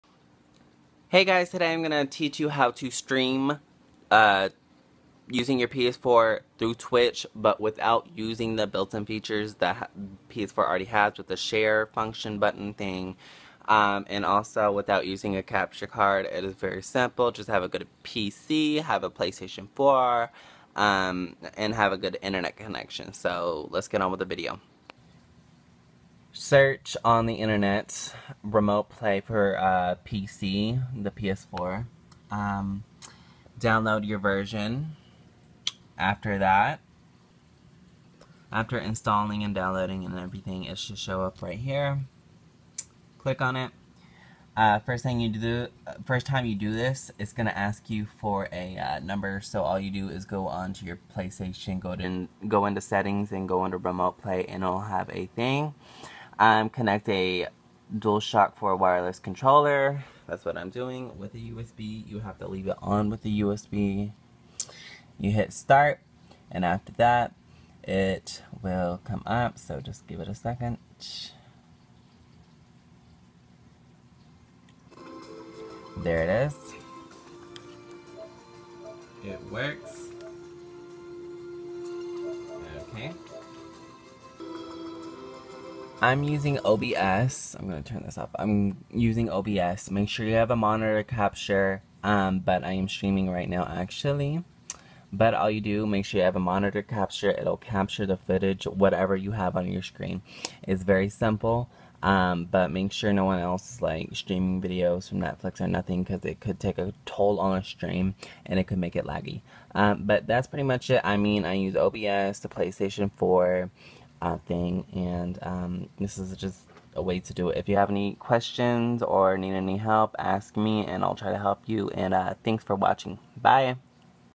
Stream ps4 on twitch using remote play
Today I teach you how to stream PS4  on twitch without capture card or using the built in streaming option through the share button.